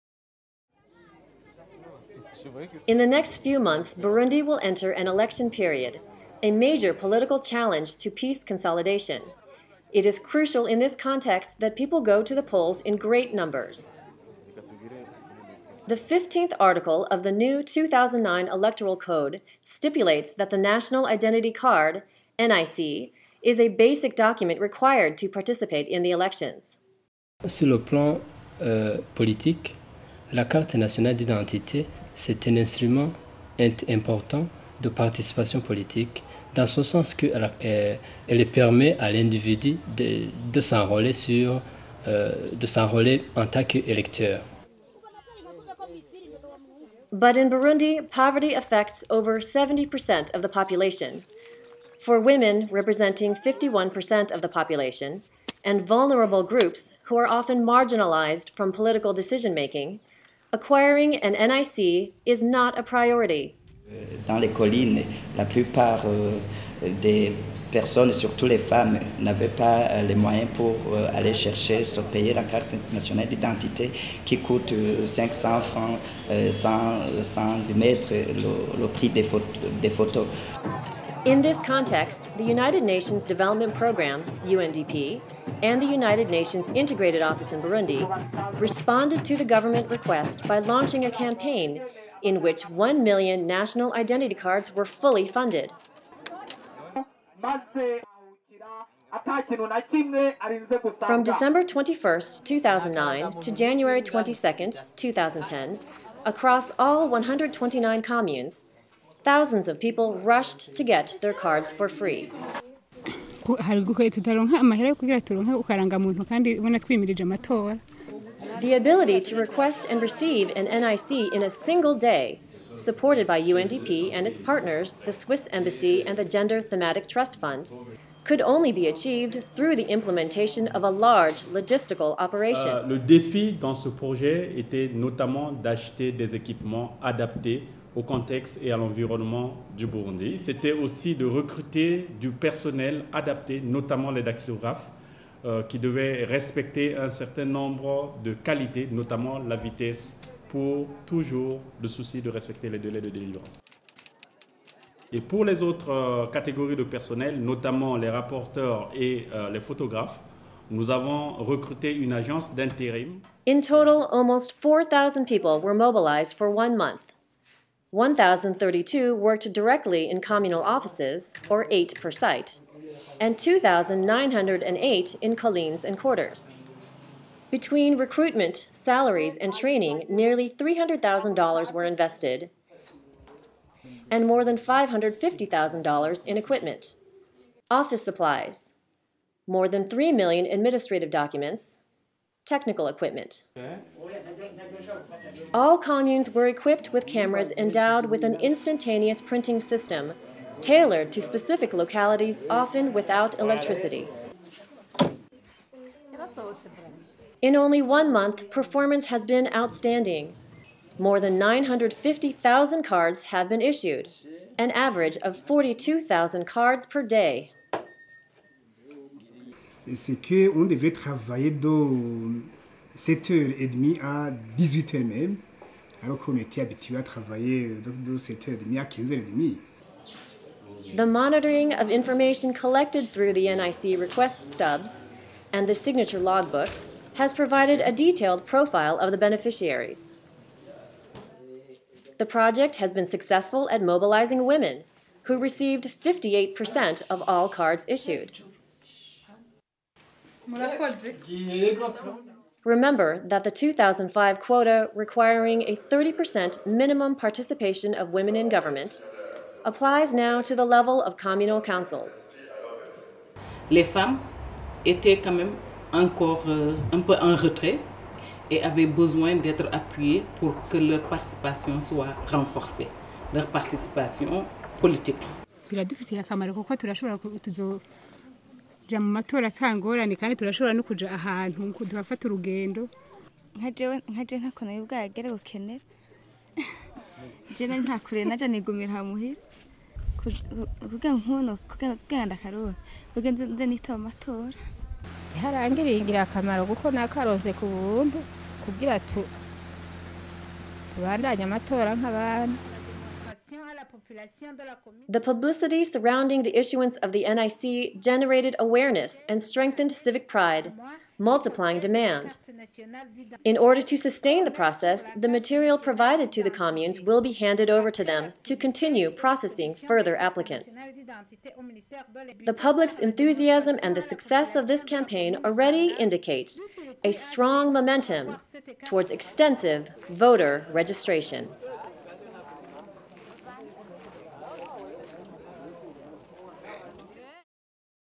Nearly One Million Identity Cards for Burundian Citizens
More than 968,000 cards and an additional almost 1 million Burundians empowered to pursue their right to vote in the upcoming electoral cycle.